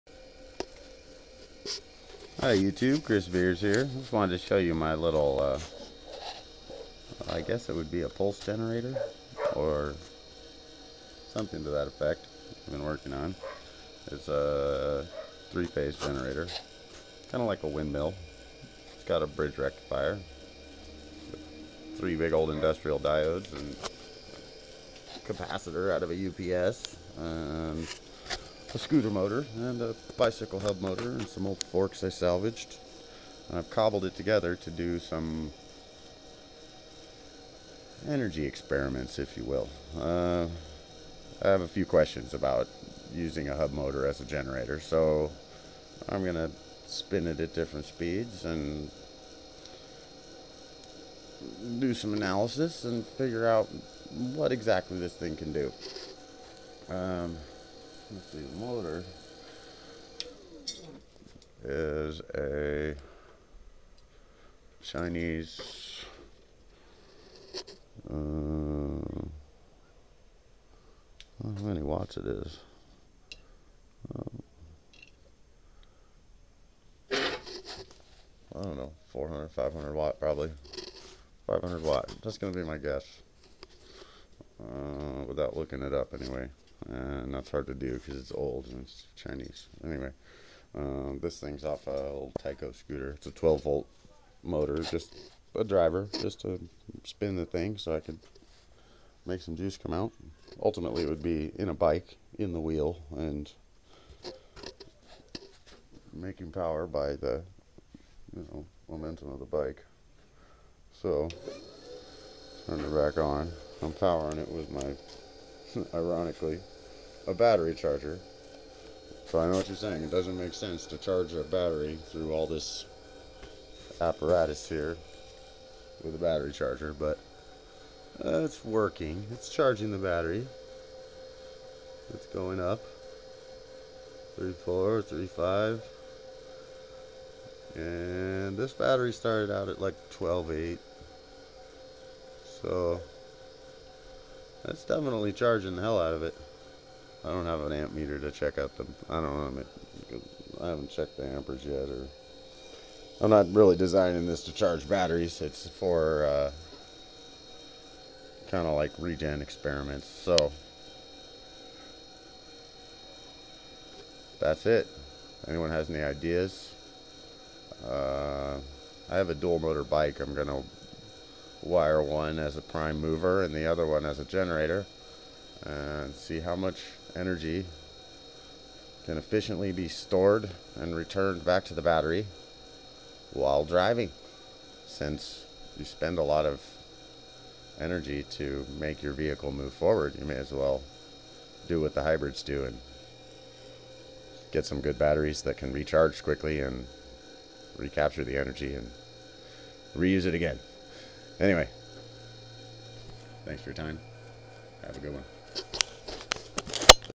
Bicycle Hub Motor PMG
I built this to experiment with more dedicated regen for my electric bike. I will use a similar system to charge the bikes battery using the momentum of myself and my bike to spin the generator rather than the energy all being wasted by the prime mover. This system makes a solid 14.4 volts plenty to drive an inverter and charger. Just need a better capacitor bank and batteries that charge as fast as they discharge.